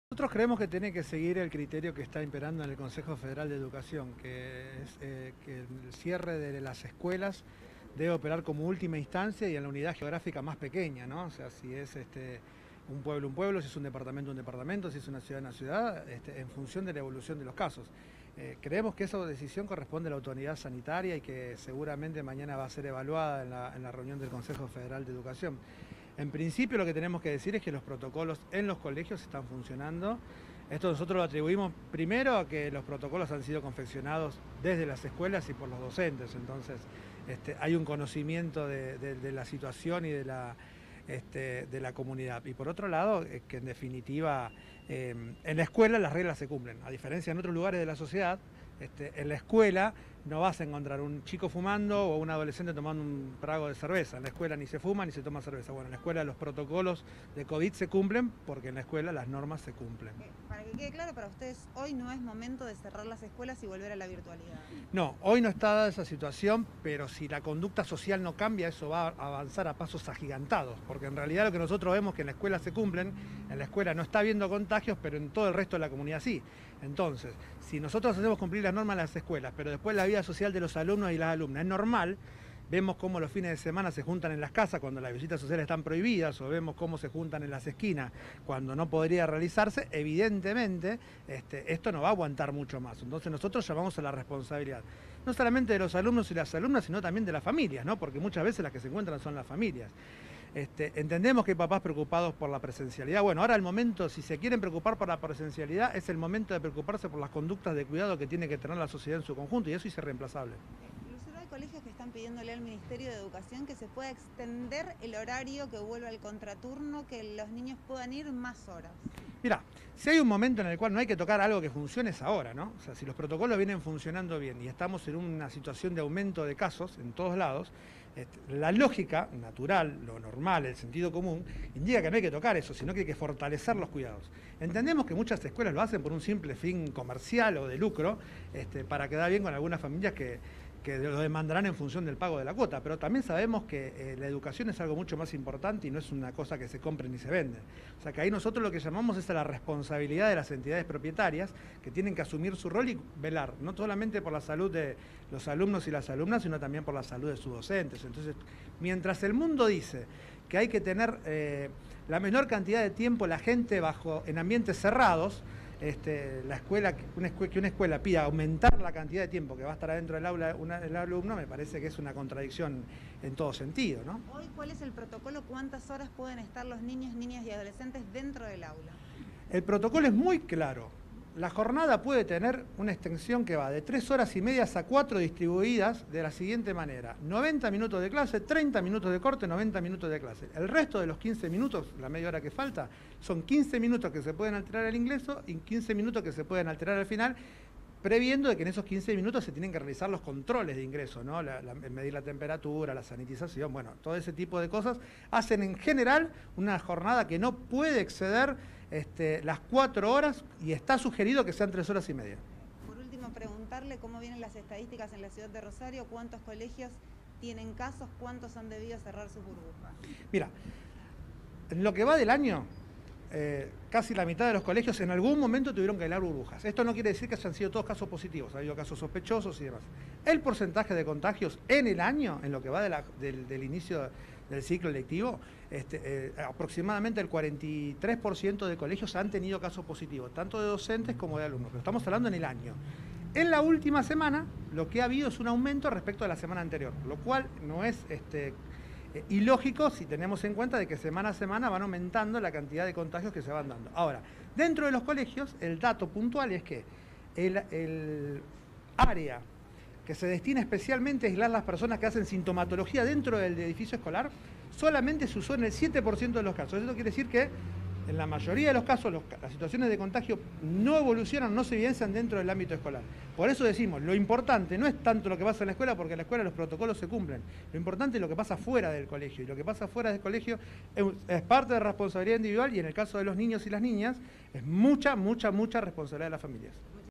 Docentes privados frente a la segunda ola. Martin Lucero SADOP Entrevista.
MARTÍN LUCERO - SADOP